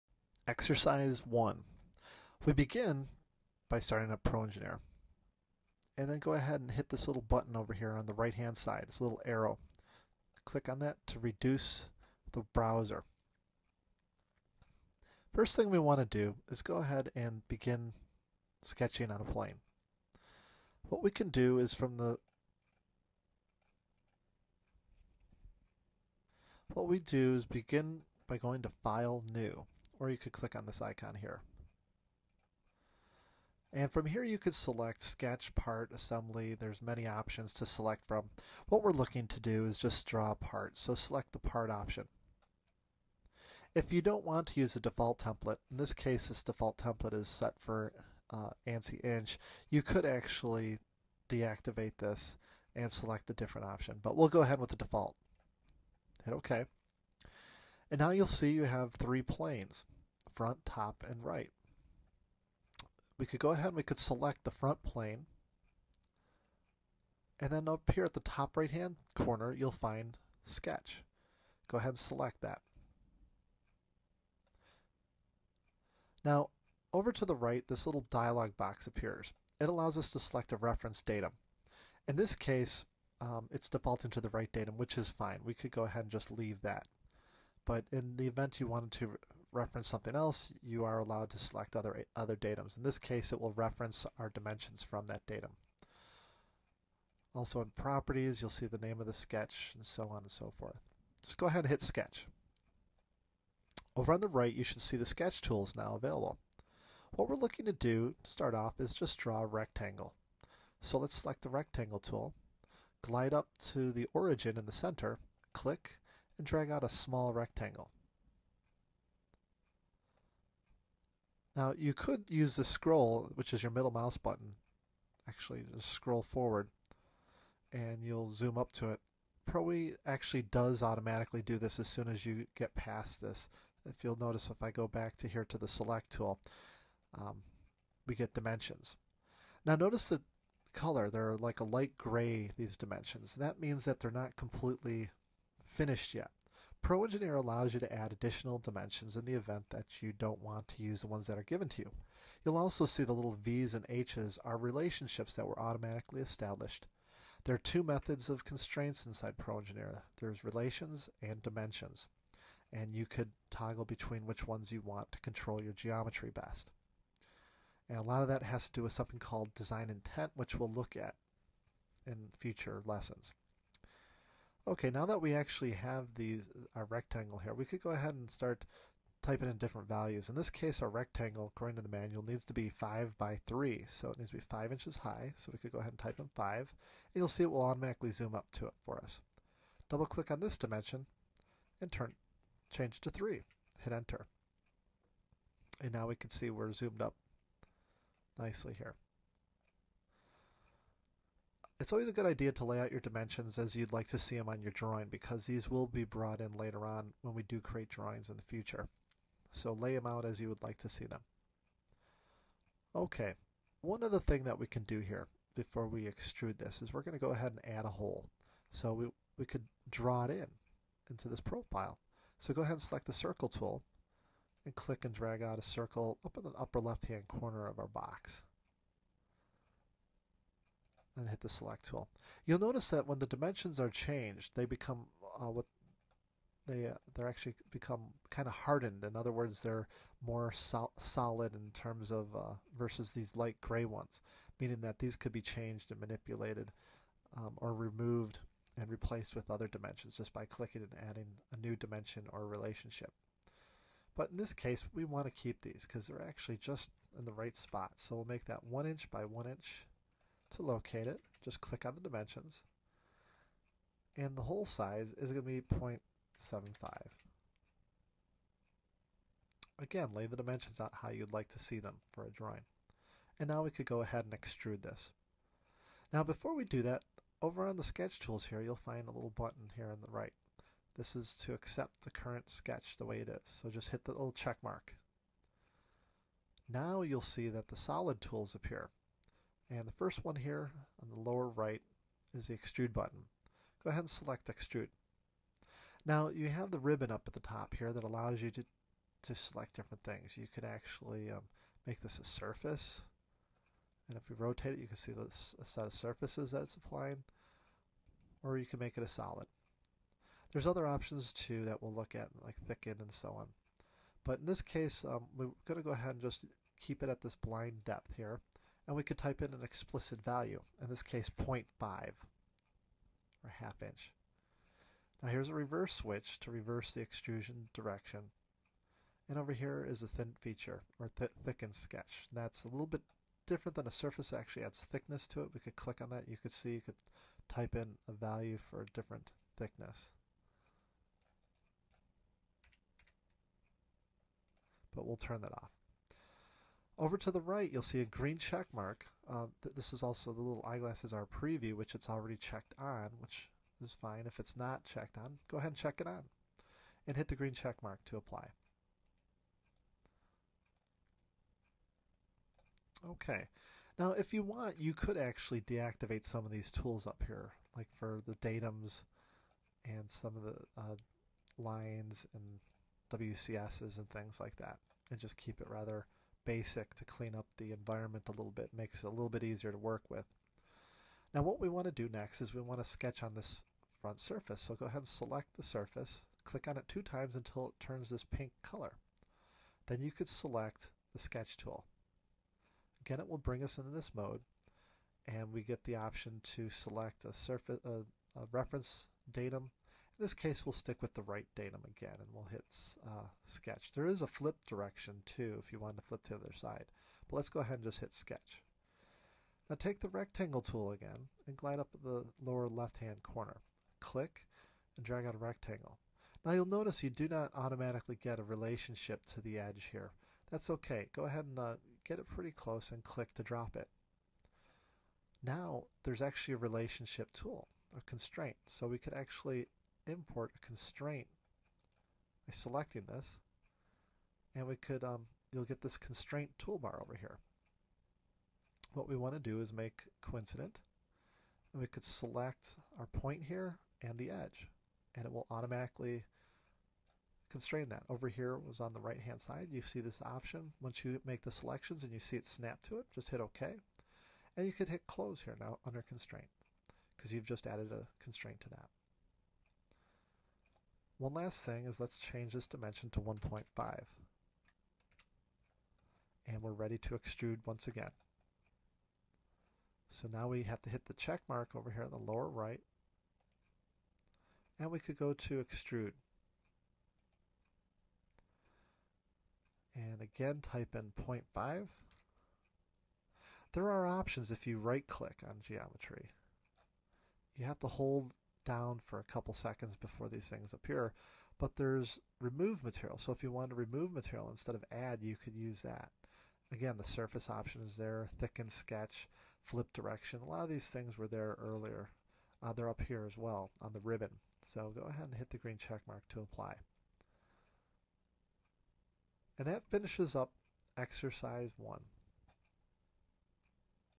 E1 Pro/Engineer Wildfire 4 - Basic Modeling 1
Exercise 1 - Introduction to the interface, basic sketching and extrusions.